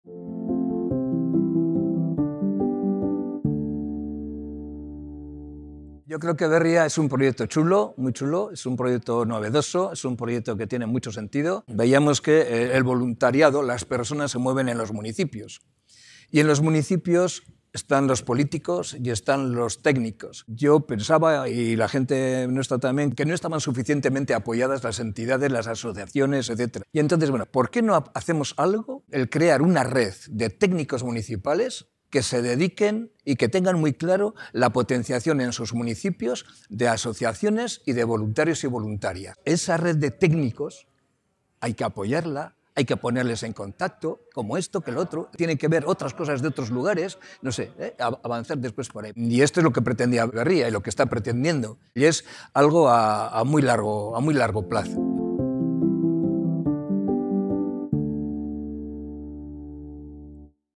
Bherriaren 5. urteurrena | Jose Alberto Vicente
[EU] Bherrian azken urteotako errepasoa eginez ospatzen dugu 5. urteurrena. Oraingoan, Jose Alberto Vicente elkarrizketatu dugu, Boluntariotzaren Euskal Kontseilua, eta Bherria-ren onuren inguruan hitz egiten du.

[ES] En Bherria celebramos nuestro 5º aniversario haciendo un repaso de los últimos años, entrevistamos a Jose Alberto Vicente, Consejo Vasco del Voluntariado, y nos habla sobre los beneficios de Bherria.